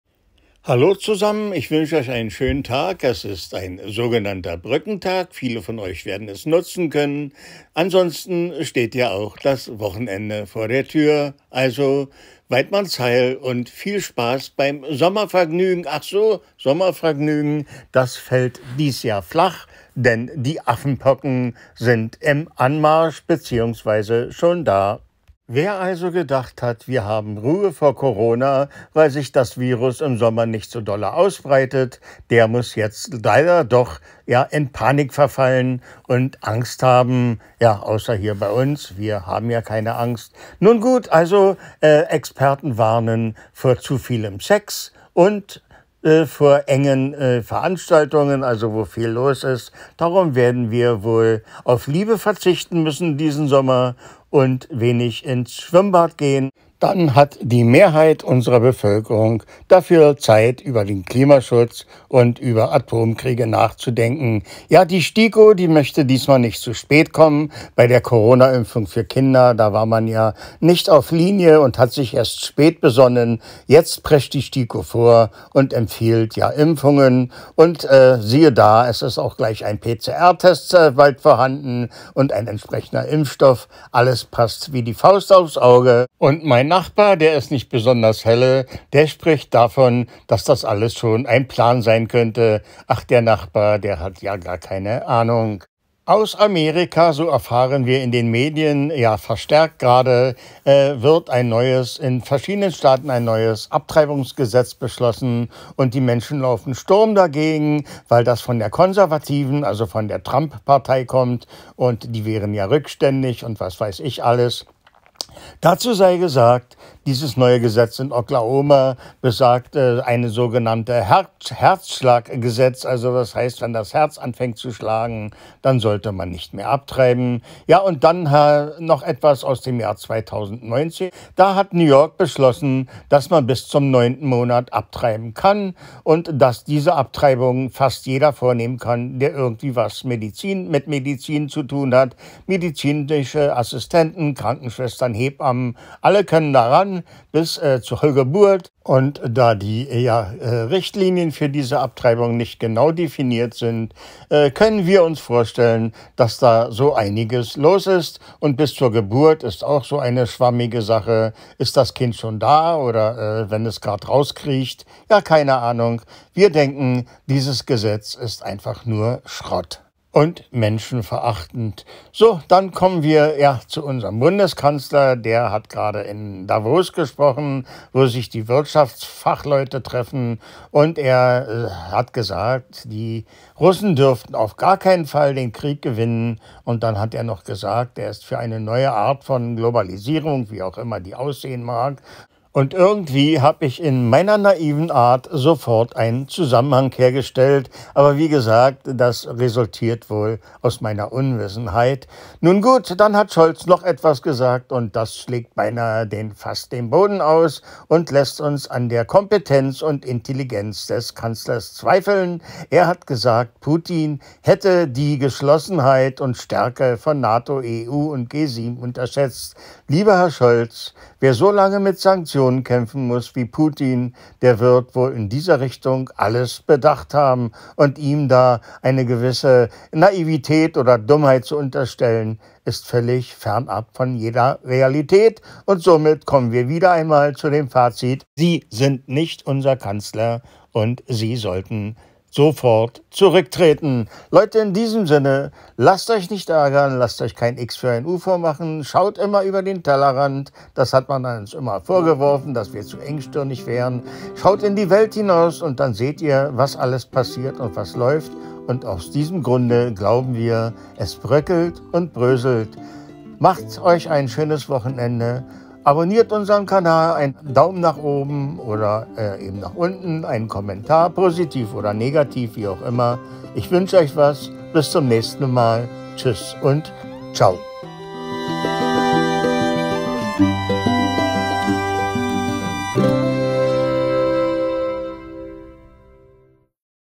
Scholz untragbar - Rücktritt unvermeidlich
Scholz hält Putin für dumm…….
Der Kanzler glänzt mit Naivität!
USA : Abtreibung bis zur Geburt!!!

Satire!!!
Wenn Sie uns unterstützen möchten:
Und IBAN:
DE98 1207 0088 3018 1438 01
Empfänger: Michael
Danke